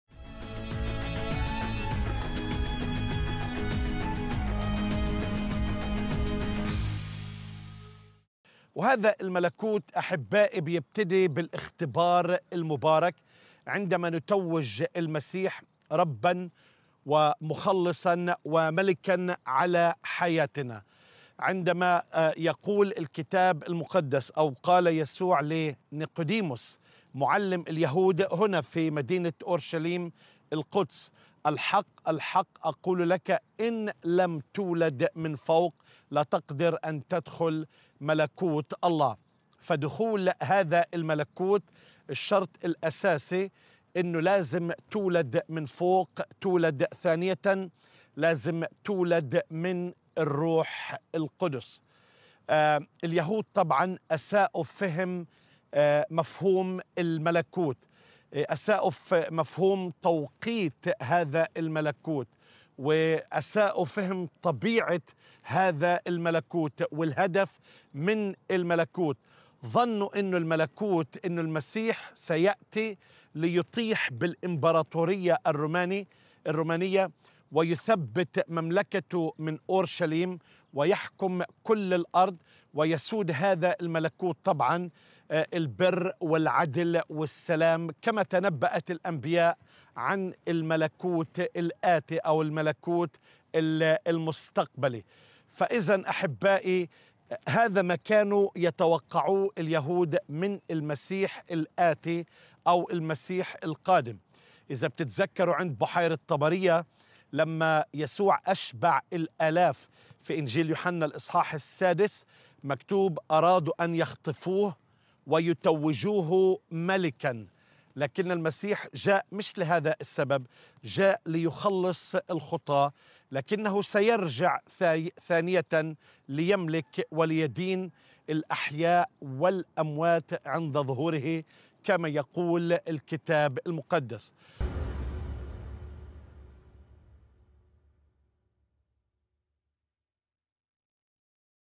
14:03 ما هو الشرط الأساسي الذي وضعه المسيح لدخول ملكوت الله؟
تشاهدون الآن
مقطع قصير من سلسلة
الحلقة (١٤) ملكوت السموات